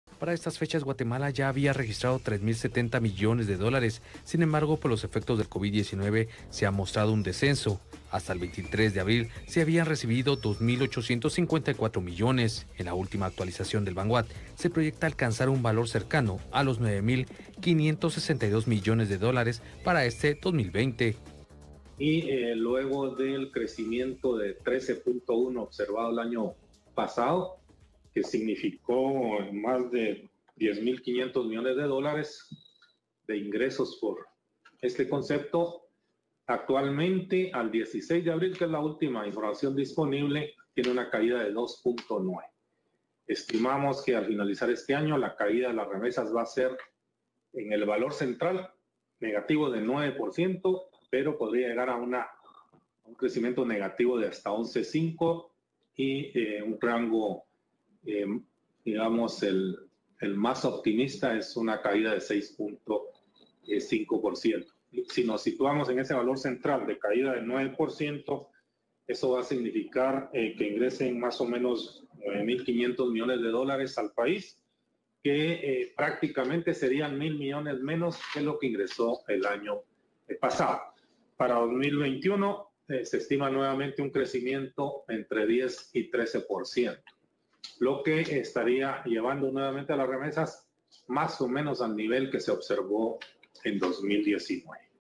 Caída en envío de remesas
Banguat confirmó la caída que enfrentarán las divisas por concepto de remesas

El Banco de Guatemala confirmó lo señalado por entidades internacionales en torno a la caída que enfrentarán las divisas por concepto de remesas.